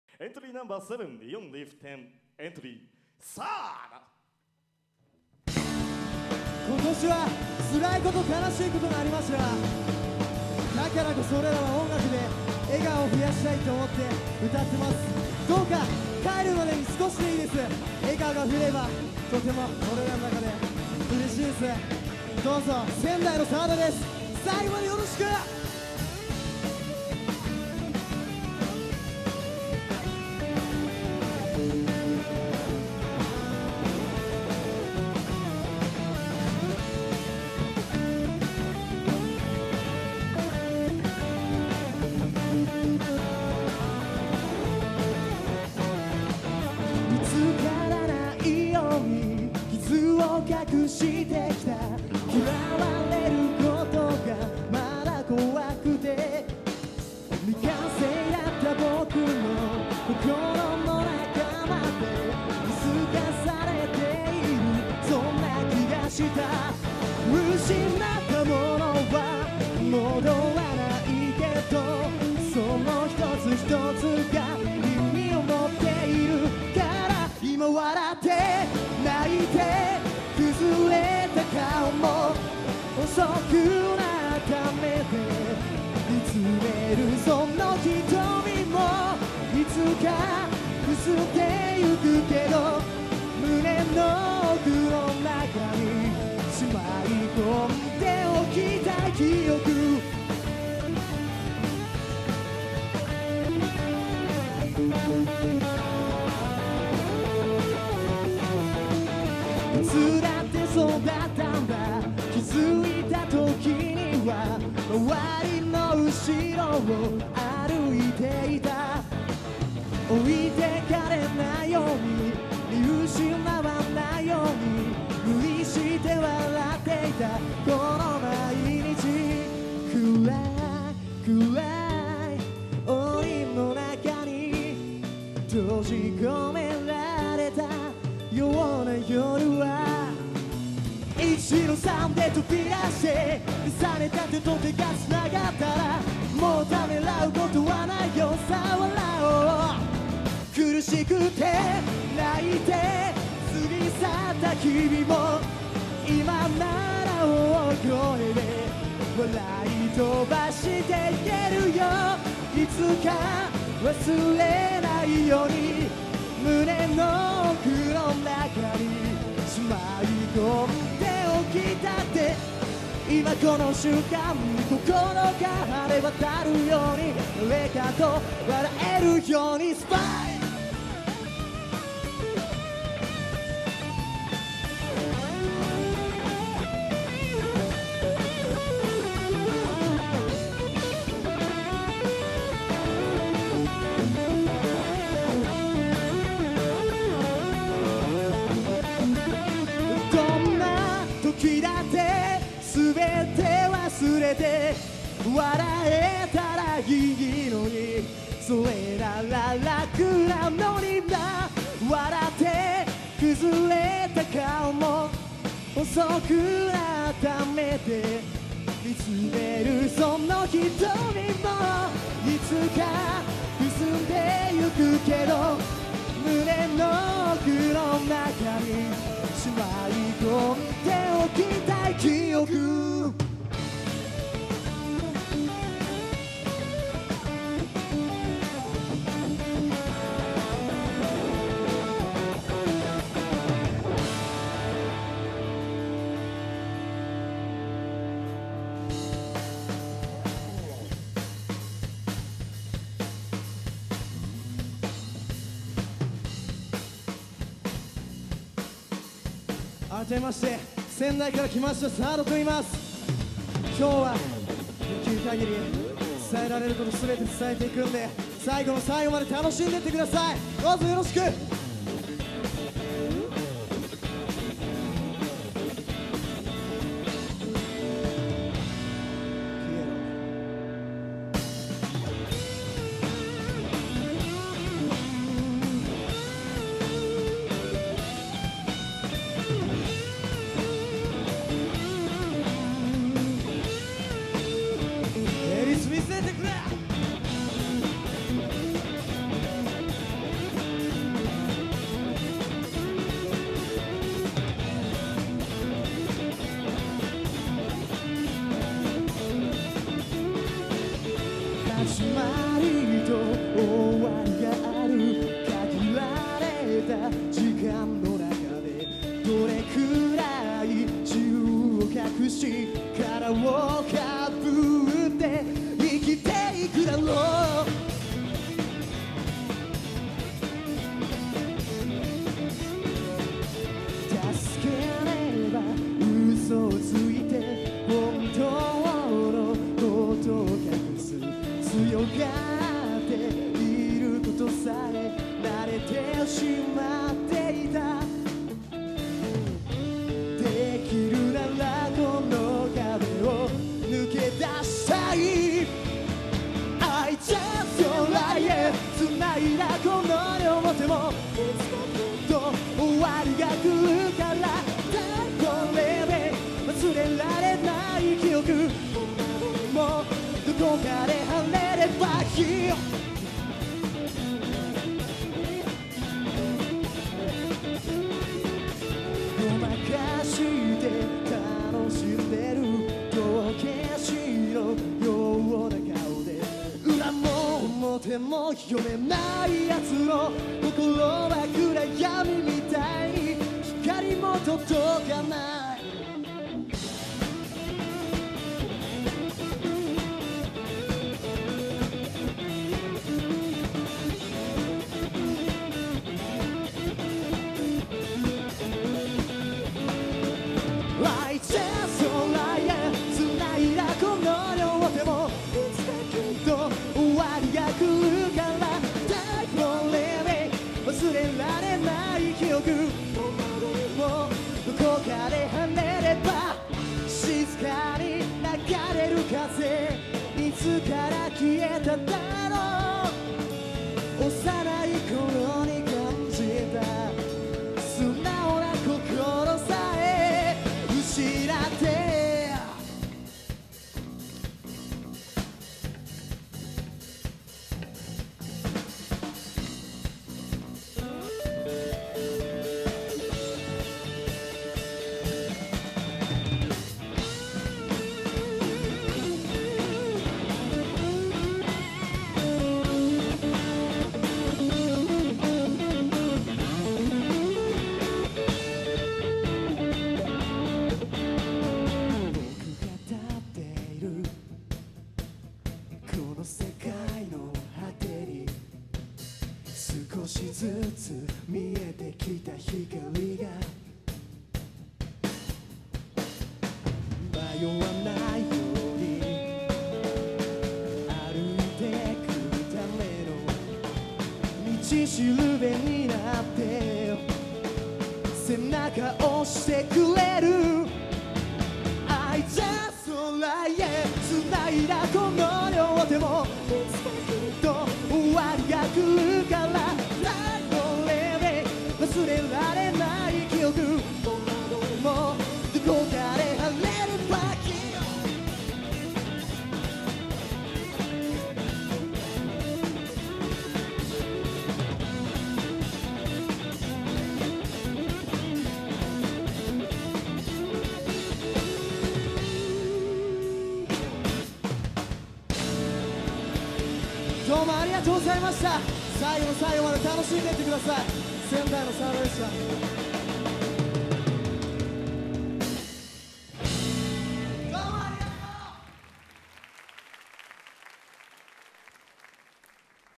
No.7 SA-D HOTLINE2011 ジャパンファイナル
HOTLINE2011 ジャパンファイナル出場、SA-D  (イオン利府店)のライブ映像です。